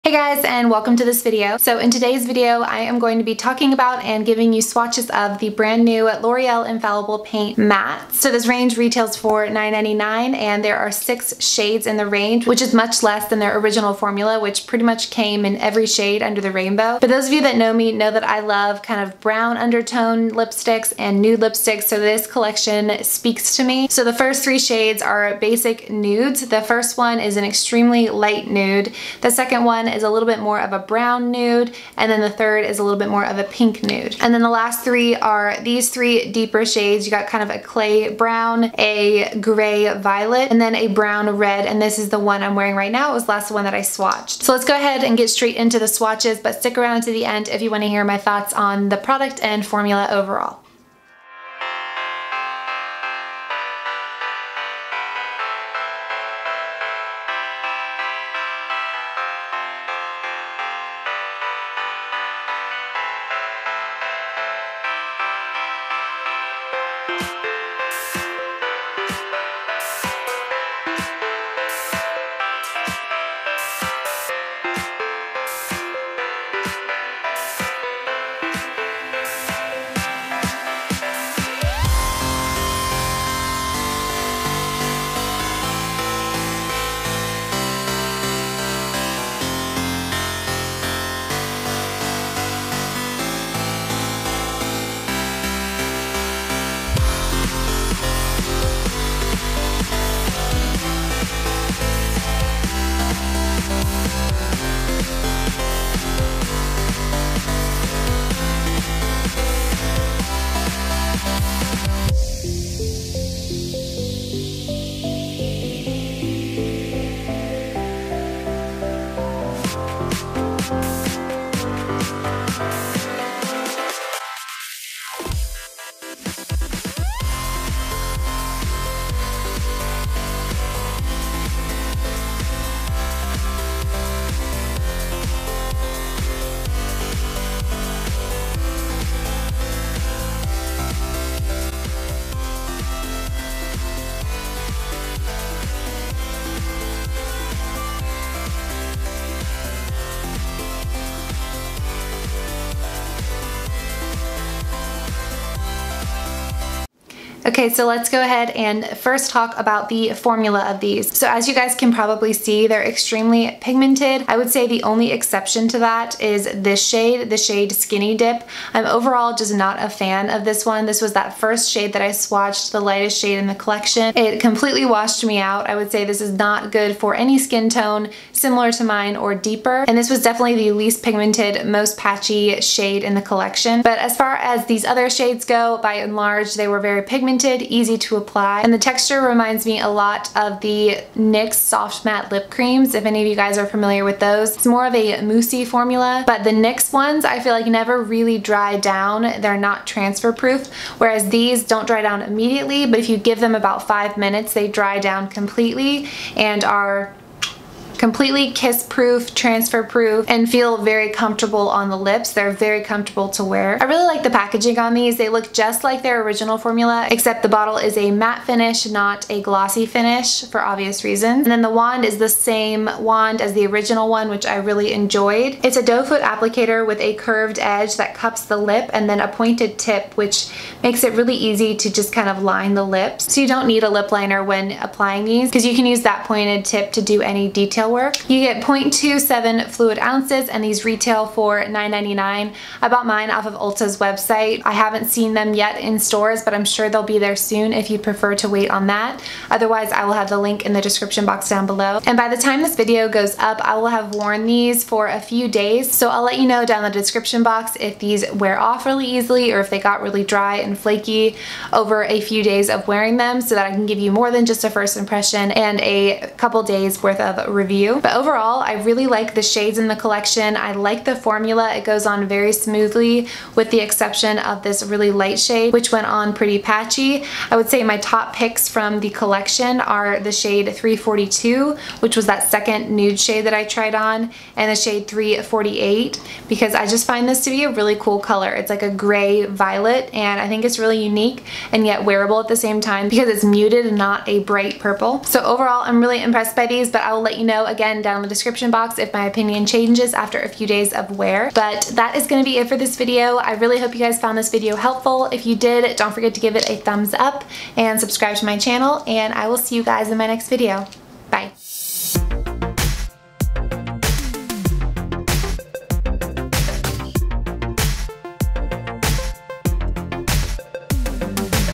NEW L'Oreal Infallible Matte Lip Paints | Lip Swatches
I hope you enjoy this swatch video of the brand new L'Oreal Infallible Matte Lip Paints! Let me know your favorite shade in the comments below.

Update: The lipstick stays on all day, even through eating, but does begin sinking into the lines of the lips and looking dry. Be careful not to apply too much product as that will make the dryness/cracking worse. Overall, I still enjoy it and plan to continue using.

PRODUCTS MENTIONED
L'Oreal Infallible Matte Lip Paints

Misha BB Cream (27)
Maybelline Fit Me Concealer (20)
Kimmel Stay Matte
Catrice Contour and Highlight
Wet n Wild Blush (Apri-cot in the middle)
L'Oreal Lash Paradise
elf Flirtatious Luxe Lashes
L'Oreal Infallible Paints - Matte Lipstick Cinna-Bomb

Nails: Revlon Gray Suede

SHADE COMPARISON
Maybelline Fit Me 128
L'Oreal Stay Matte 105
Maybelline Better Skin 30
L'Oreal True Match W4